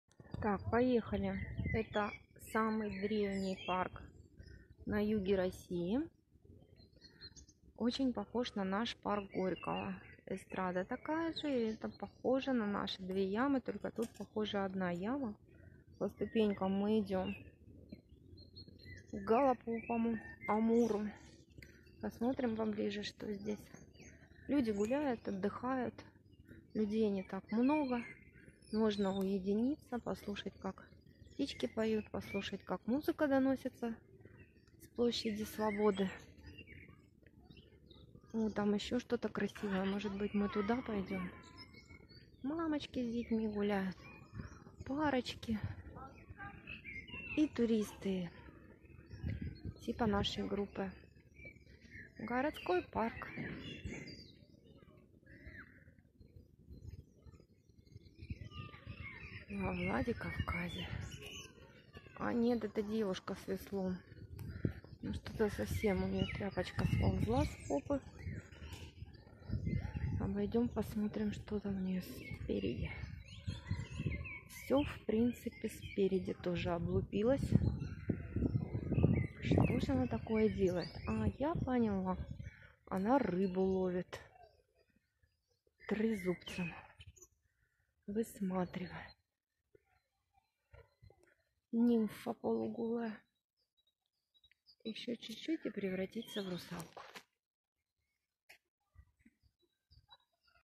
Фонтан с "русалочкой".
Алания (Северная Осетия). Апрель 2022. Парк Коста Хетагурова. Продолжение променада.